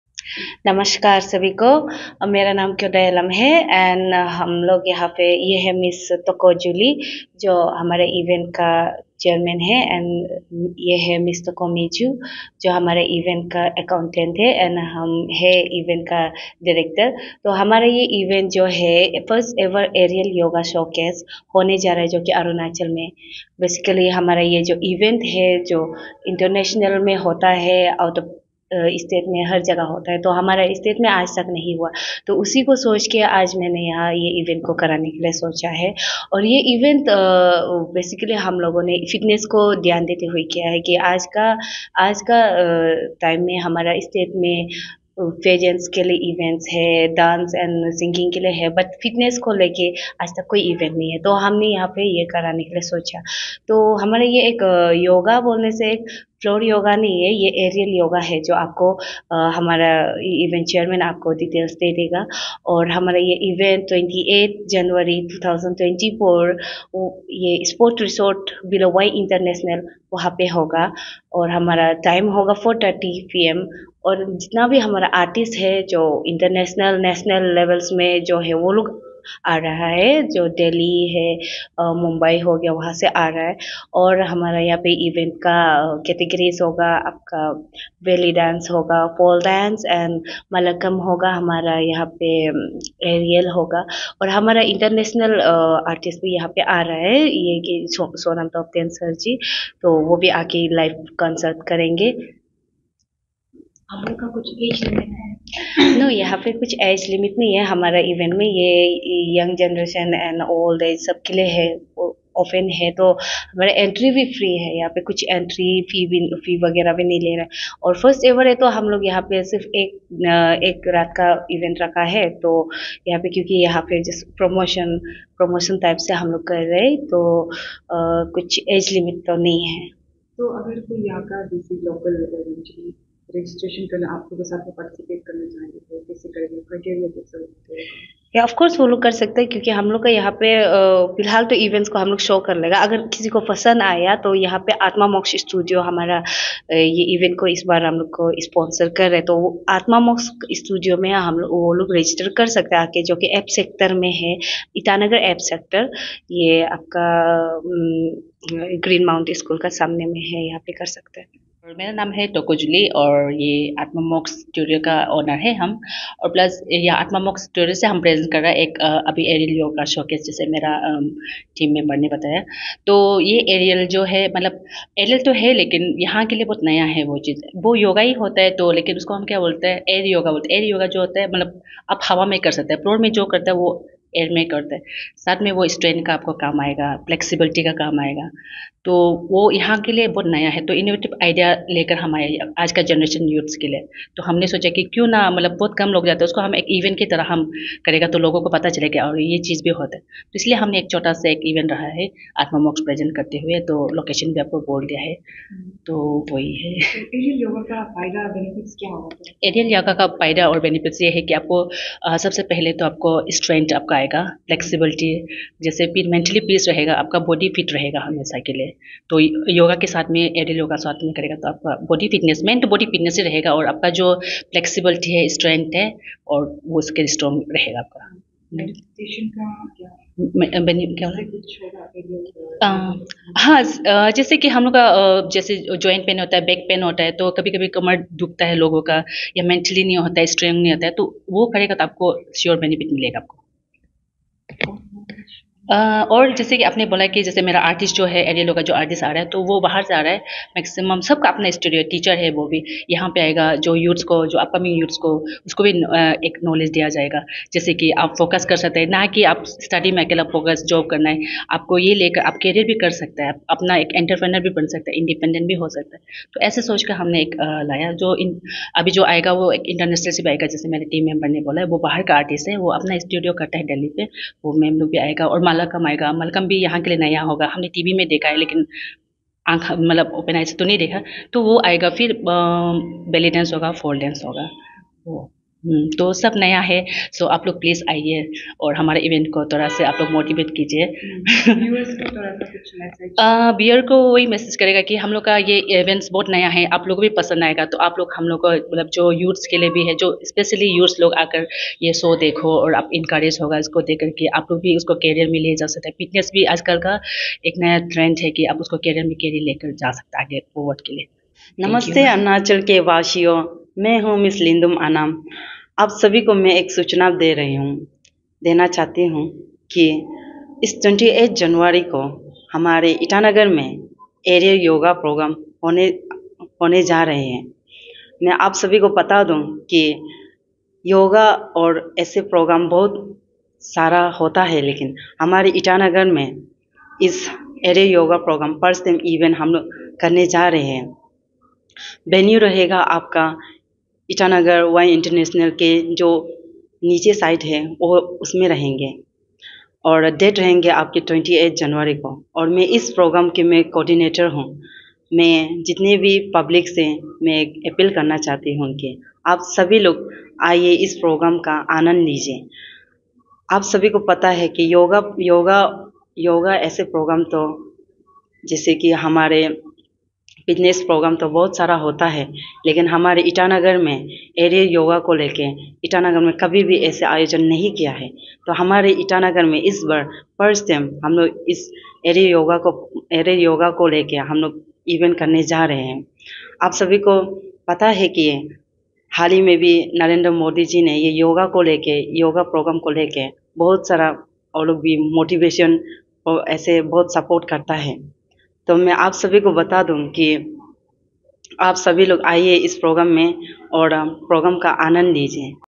First ever Aerial Yoga Show in Itanagar
First ever Aerial Yoga Show to be held in Itanagar on 28 January. The show shall have performance from international yoga trainers.